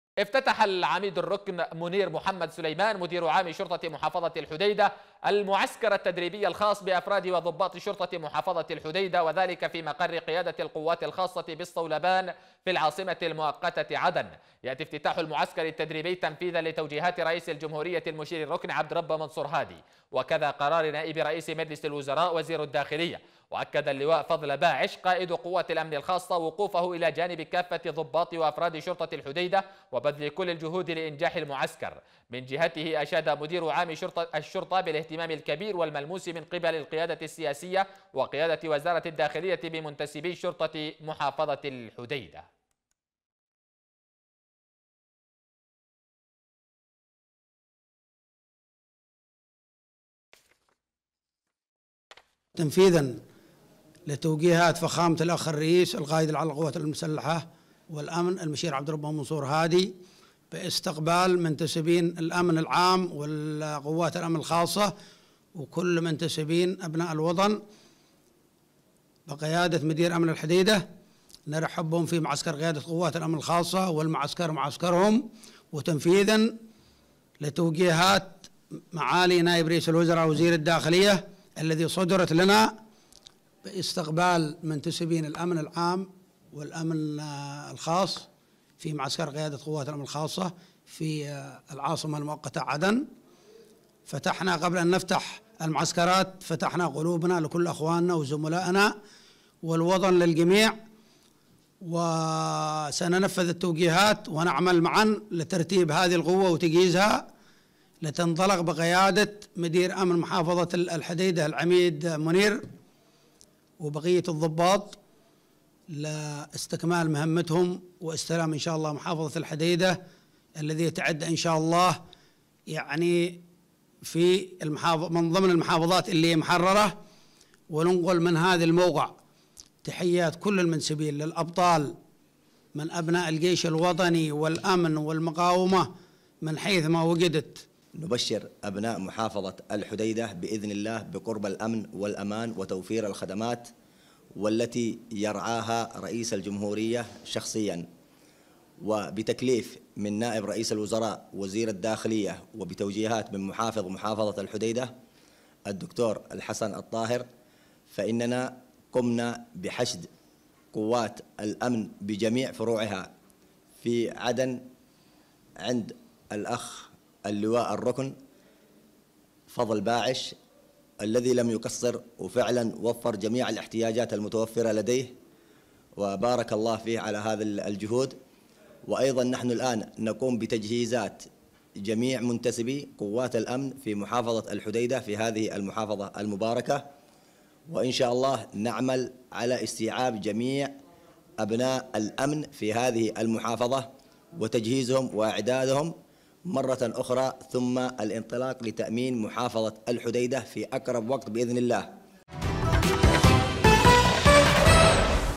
عدن.. الداخلية وشرطة الحديدة تفتتحان المعسكر التدريبي لمنتسبيها
التردد الجديد للقناة على مدار النايلسات (11603 افقي H ) افتتح العميد الركن/ منير محمد سليمان مدير عام شرطة محافظة الحديدة المعسكر التدريبي الخاص بأفراد وضباط شرطة محافظة الحديدة وذلك في مقر قيادة القوات الخاصة بالصولبان في العاصمة المؤقتة عدن .
ويأتي افتتاح المعسكر التدريبي  تنفيذا لتوجيهات رئيس الجمهورية المشير الركن/ عبد ربه منصور هادي ، وقرار نائب رئيس مجلس الوزراء وزير الداخلية .
وأكد اللواء/ فضل باعش قائد قوات الأمن الخاصة وقوفه الى جانب كافة ضباط وافراد شرطة الحديدة وبذل كل الجهود لإنجاح المعسكر.